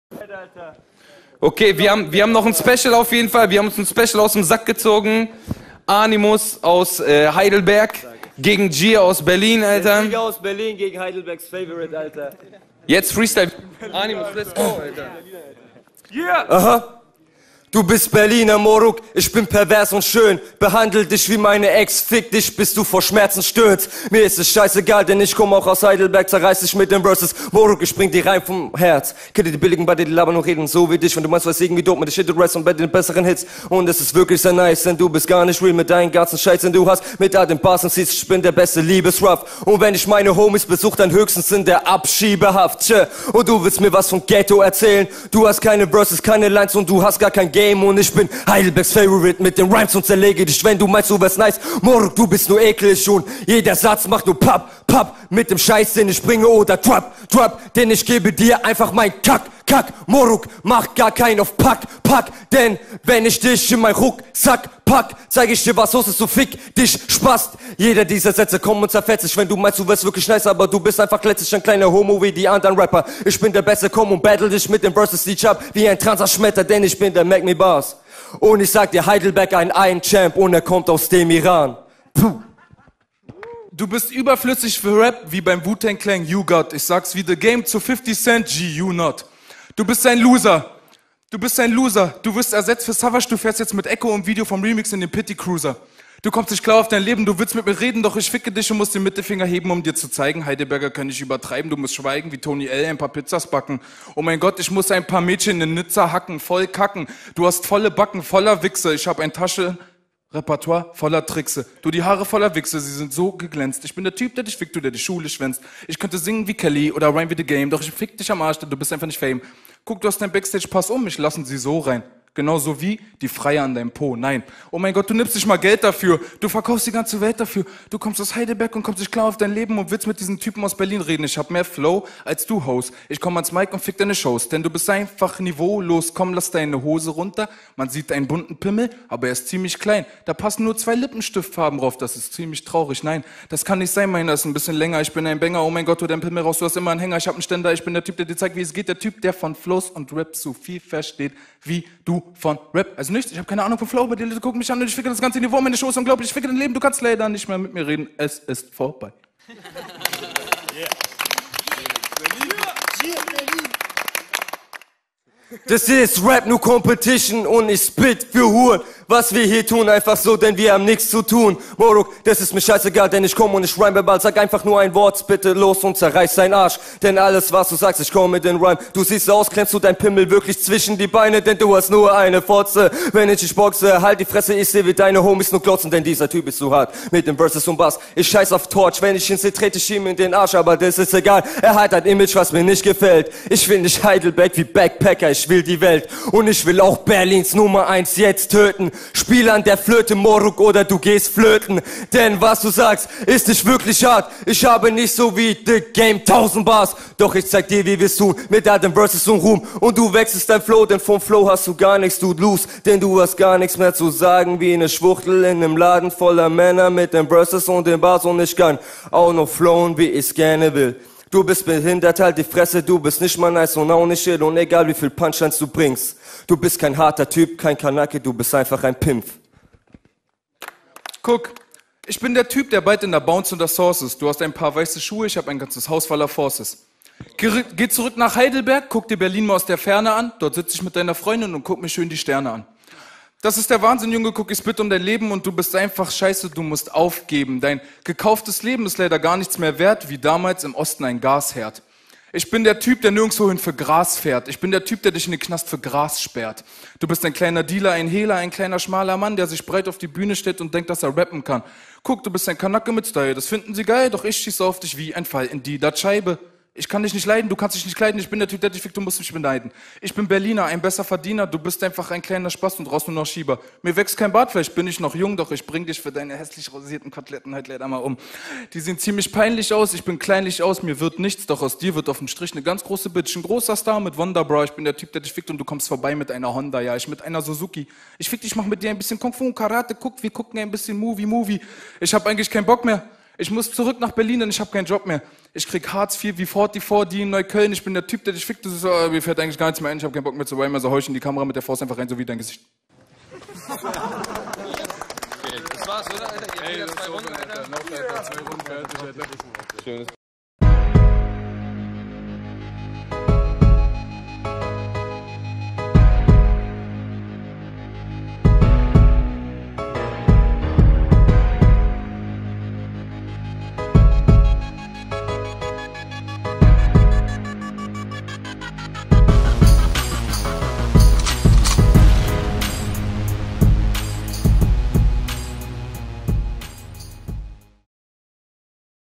Animus vs. Gier - Feuer über Deutschland 1 SPECIAL (hosted by Kool Savas)
►Animus (Team AufDieFresse)
►Gier (1ON1 Allstars Team)